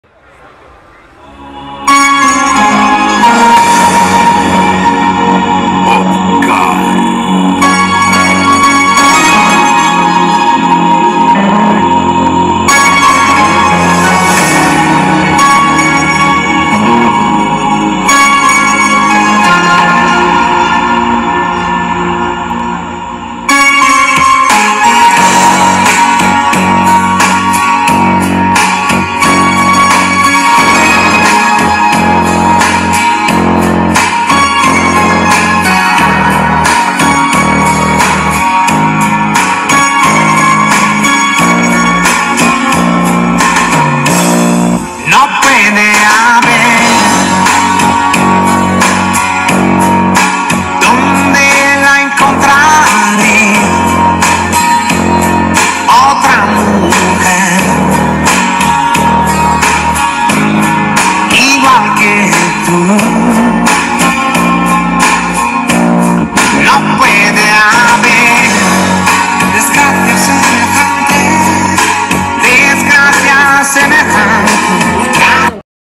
COMPETENCIA CAR AUDIO FINAL NACIONAL OPCAR / LARA FEST 2019 DSC 0228
OPCAR con lo mejor de la FINAL NACIONAL OPCAR 2019 que se llevo a cabo en Barquisimeto en el Complejo Bicentenario

& TUNING

Competencias de JDM, Motos de Alta Cilindrada,

Recuerden Difundir